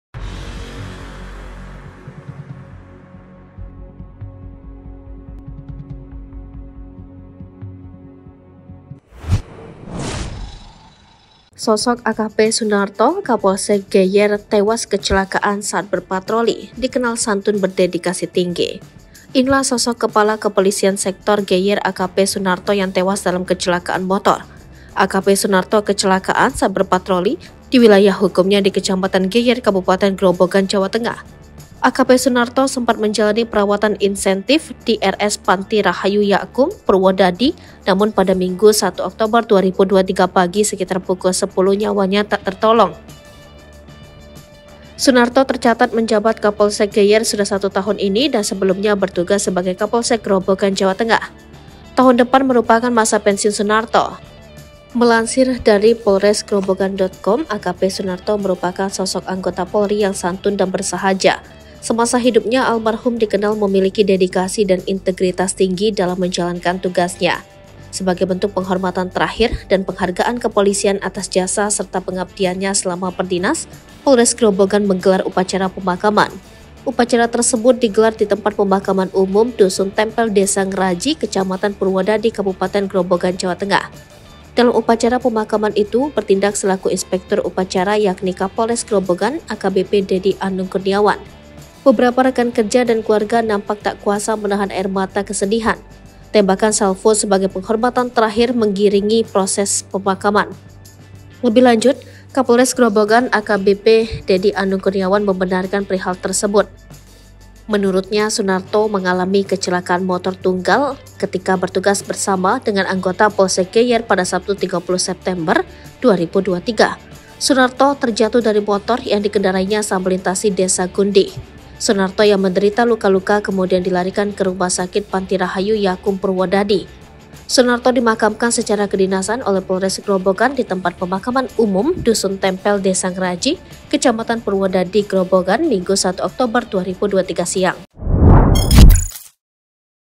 Sosok AKP Sunarto Kapolsek Geyer Tewas Kecelakaan Saat Berpatroli, Dikenal Santun Berdedikasi Tinggi
Inilah sosok Kepala Kepolisian Sektor (Kapolsek) Geyer AKP Sunarto yang tewas dalam kecelakaan motor.

AKP Sunarto kecelakaan saat berpatroli di wilayah hukumnya di Kecamatan Geyer, Kabupaten Grobogan, Jawa Tengah.

AKP Sunarto sempat menjalani perawatan insentif di RS Panti Rahayu Yakkum Purwodadi, namun pada Minggu (1/10/2023) pagi sekitar pukul 10.00 nyawanya tak tertolong.
Yuk lihat informasi terkini bersama kami, di:

Palembang.tribunnews.com

Editor : sarah
Sumber : Polres Grobogan Multimedia